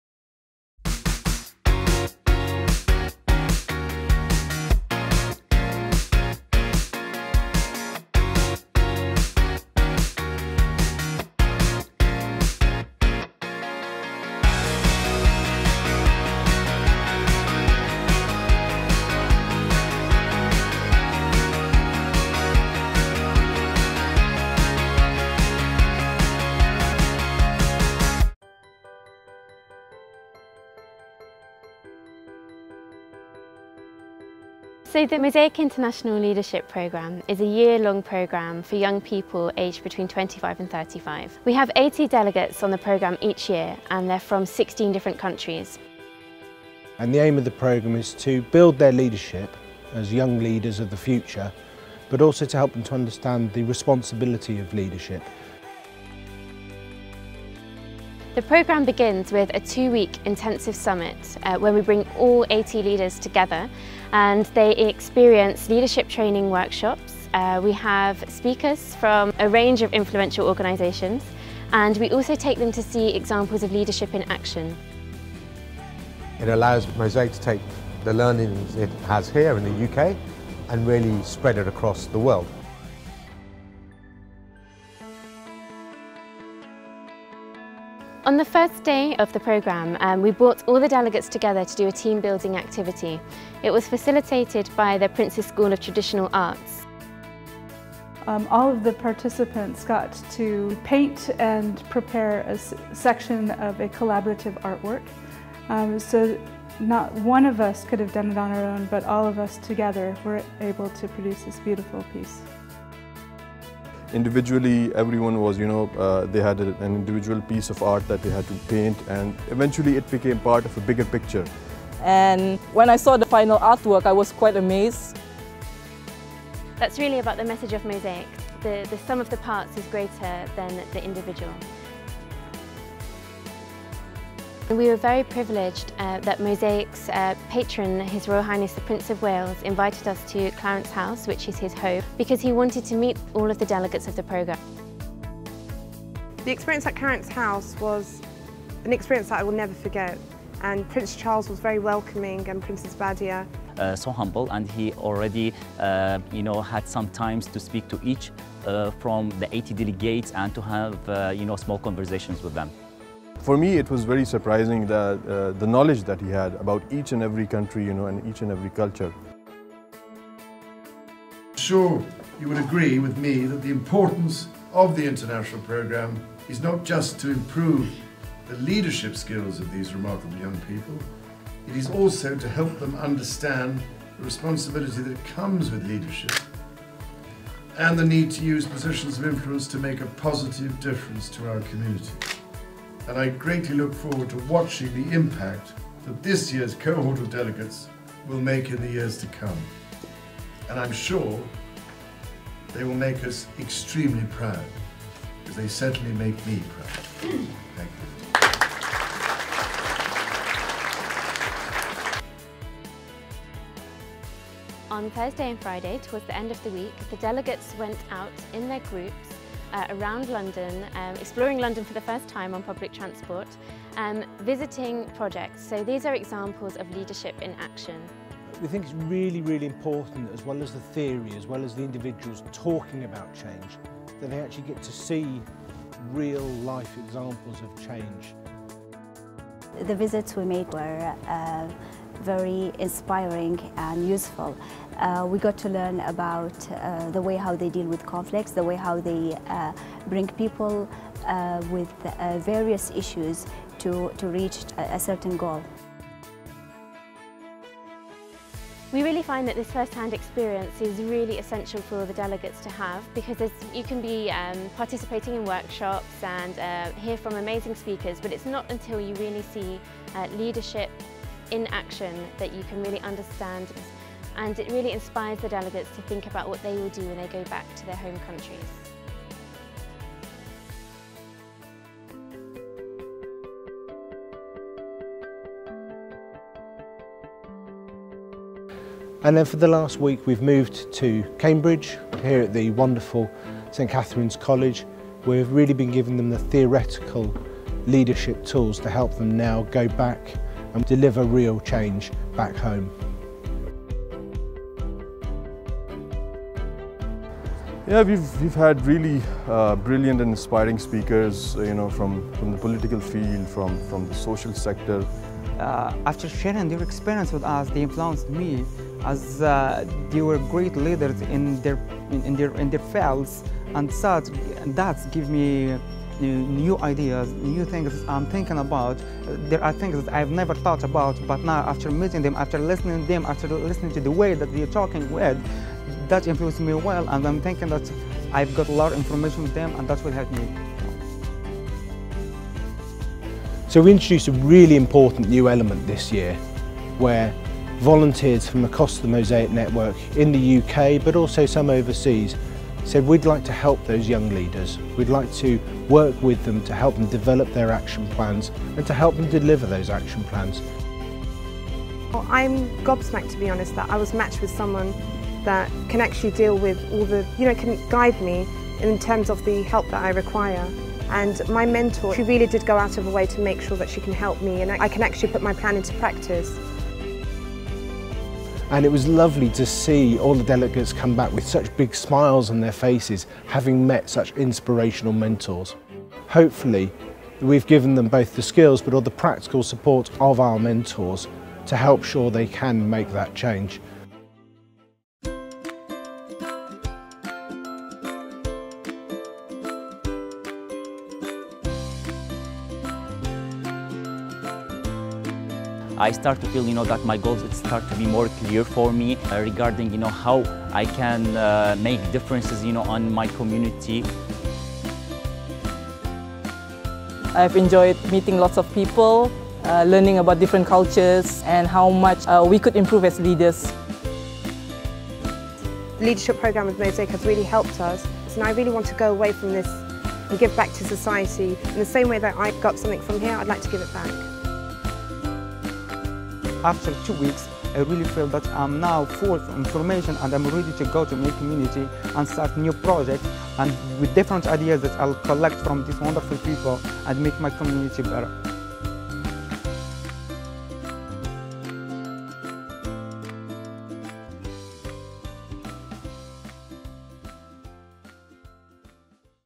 Mosaic International Leadership Programme 2013
The Mosaic International Leadership Programme aims to develop leadership skills, inspire thinking about global issues, and equip young people to become involved in their local communities. The programme lasts for 12 months, with 80 international delegates aged 25-35 taking part each year.

The programme begins by gathering the 80 delegates together for the Mosaic International Summit, a period of intense residential training, featuring world-renowned leadership trainers and speakers. The Summit includes leadership skills workshops, inspirational sessions on global issues such as poverty and sustainability, and visits to projects in the local area which demonstrate leadership in action.

The 2013 International Leadership Programme summit was hosted in London.